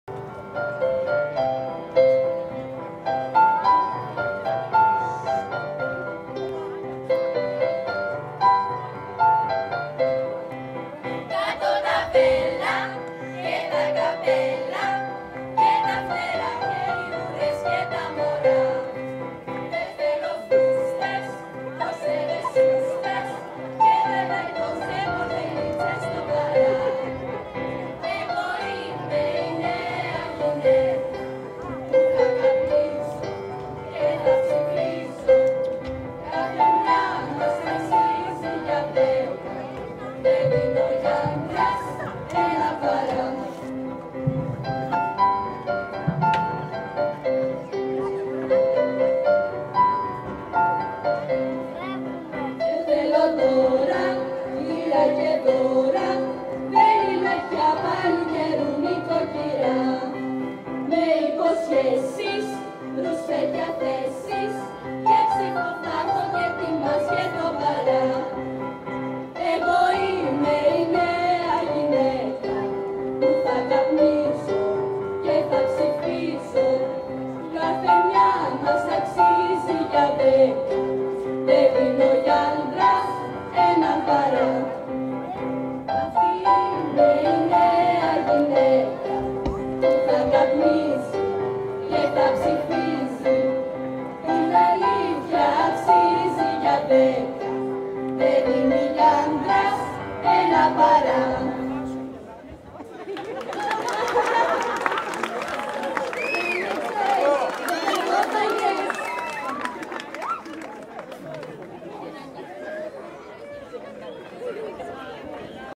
Καλλιτεχνικό Σχολείο Ηρακλείου | Ιούνιο 2010
"H γυναίκα στον 20ο αιώνα"
Καλοκαιρινή γιορτή, μαζί με το Μουσικό Σχολείο